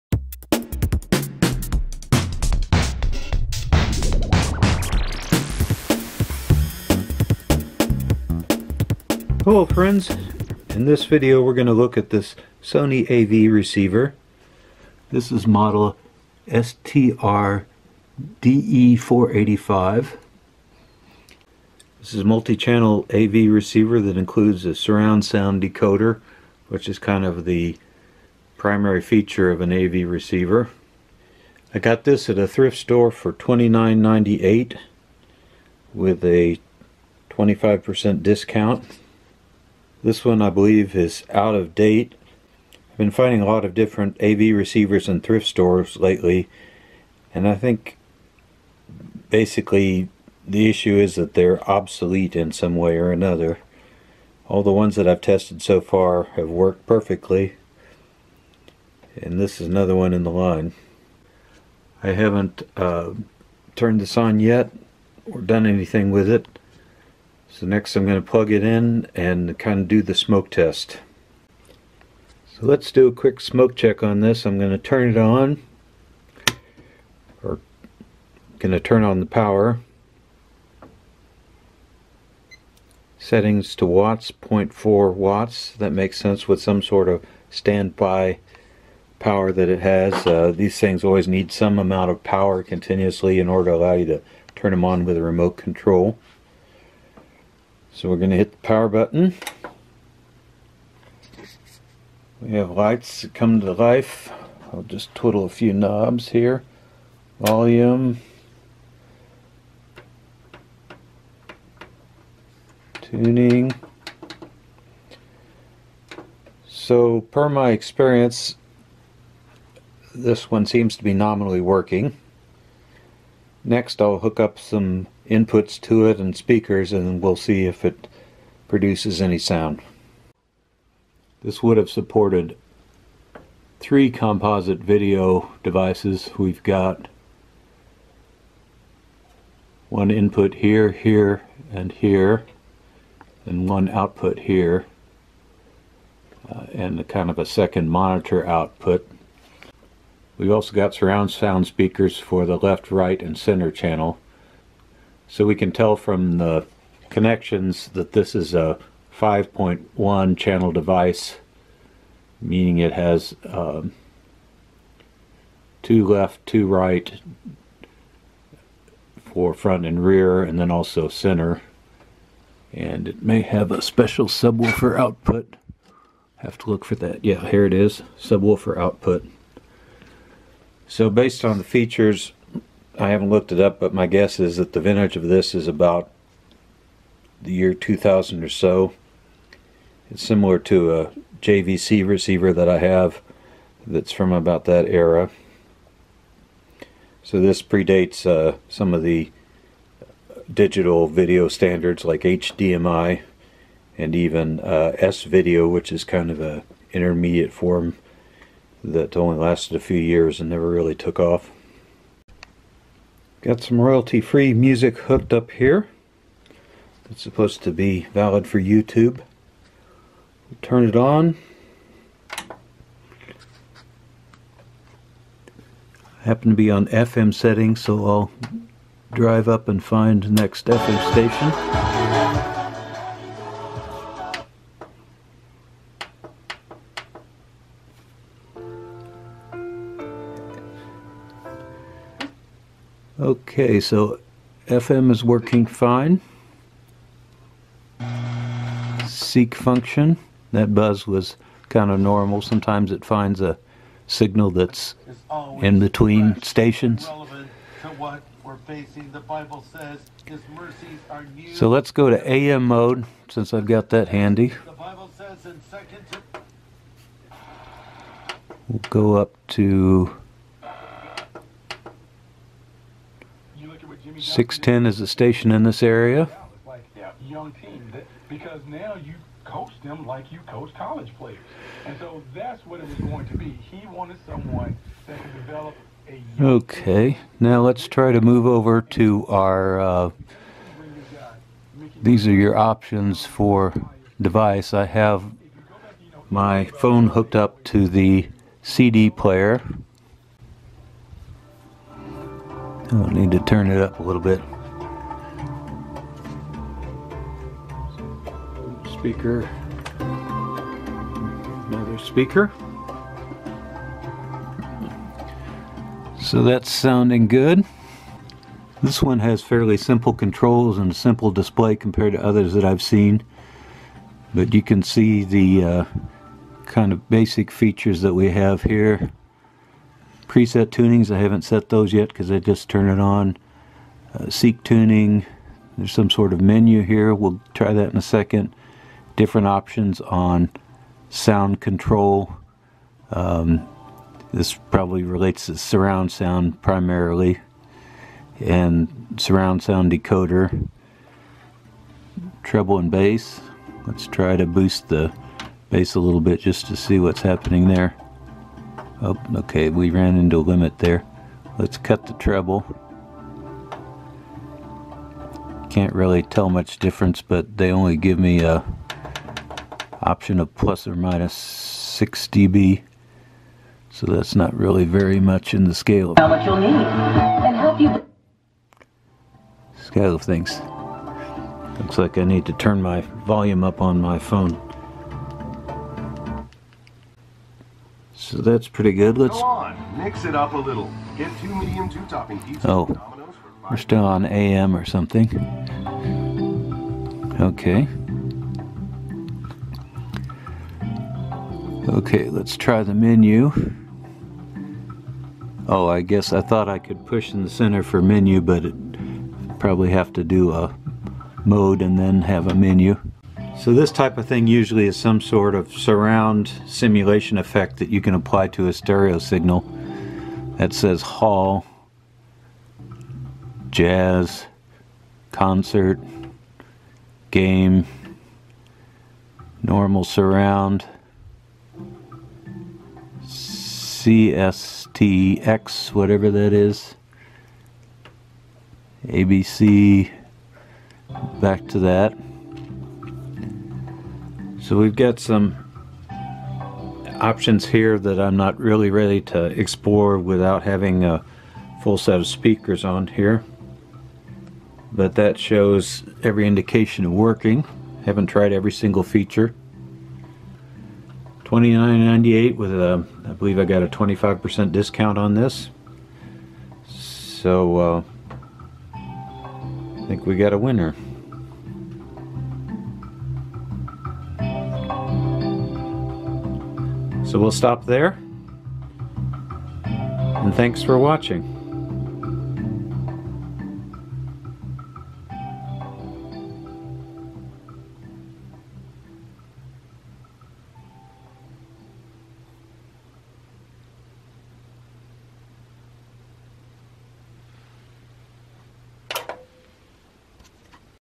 Discovering a Sony STR-DE485 AV Receiver
I test and evaluate a Sony STR-DE485 AV Receiver that I bought for cheap at a thrift store.